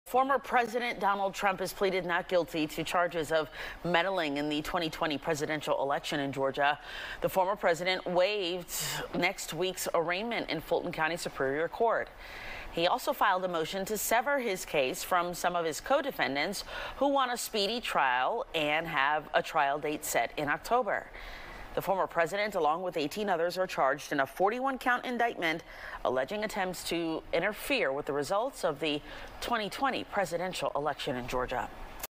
Trump pleads not guilty in Georgia election interference case | FOX 5 News
Former President Donald Trump has pleaded not guilty in the Georgia election interference case. He has also waived his right to a first appearance.